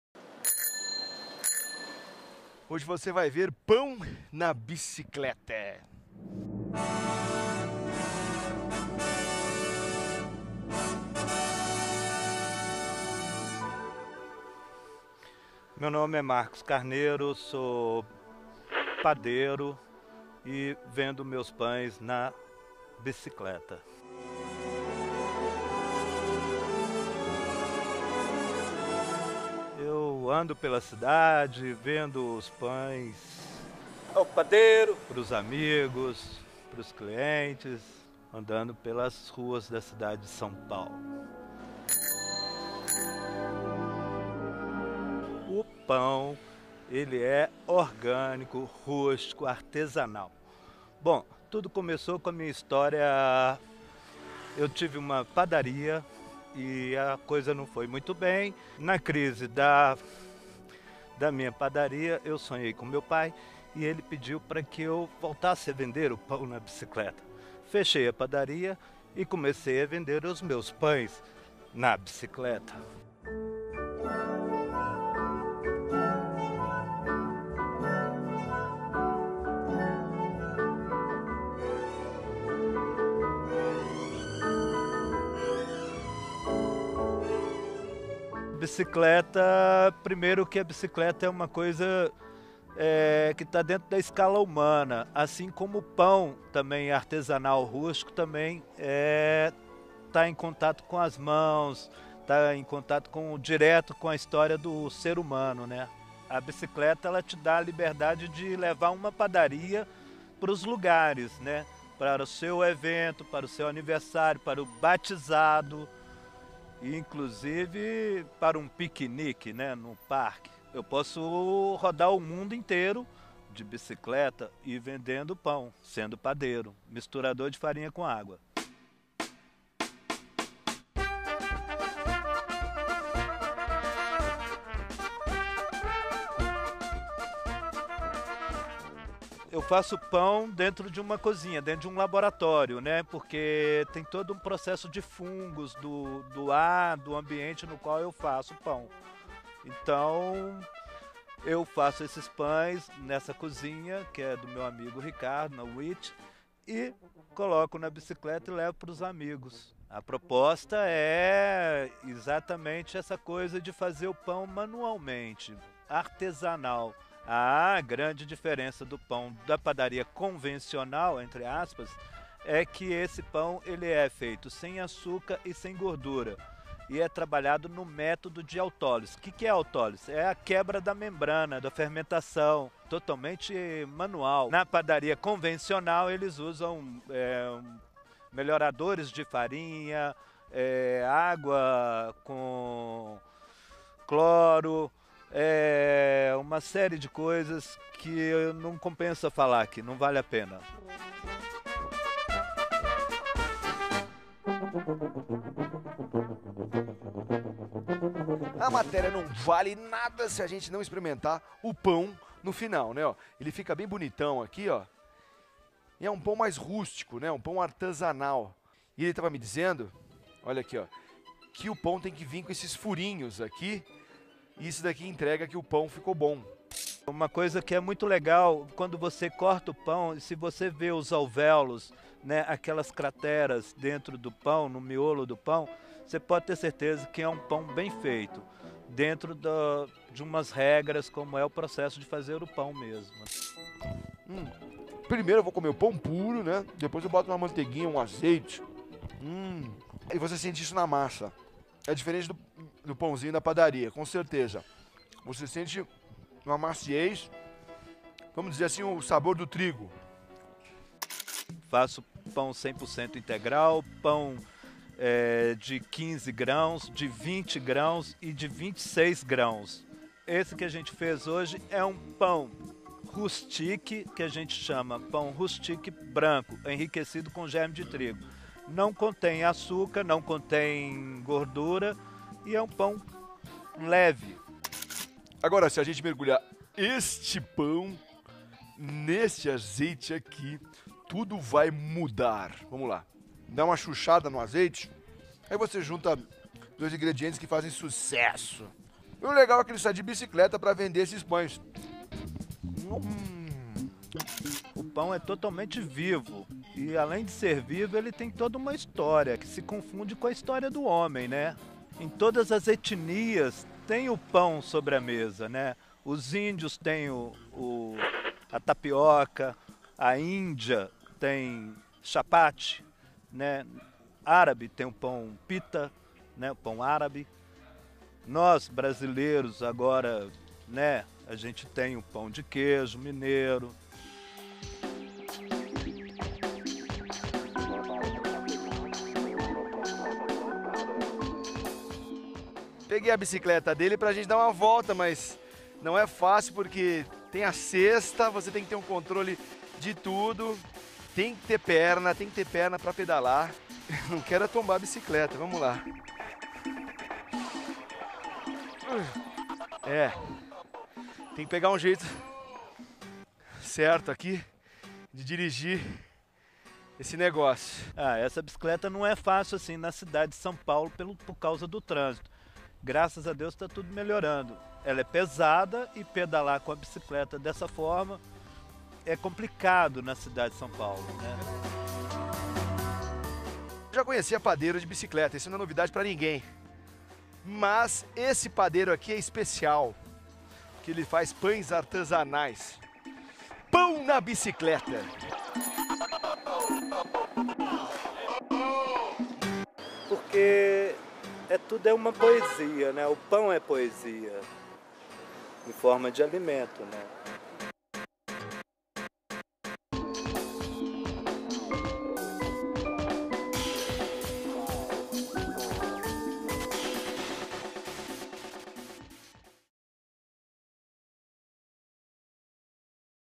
PÃO NA BIKE
Quem quer pão?
Serra da Cantareira abriga uma padaria extremamente rústica, e o cheiro de pão fresco é inebriante. O sabor do pão feito com farinha orgânica e fermento selvagem do padeiro Marcos Carnero é uma viagem aos sentidos. Ele já trabalhou ao lado de Olivier Anquier e Emmanuel Bassoleil, atualmente em sua padaria ele utiliza farinha 100% integral e levain, um processo de fermentação que pode durar por até 8 horas,  e que recebe um cheiro característico da Serra da Cantareira, conferindo um sabor mais ácido ao pão.

Mais sobre o assunto?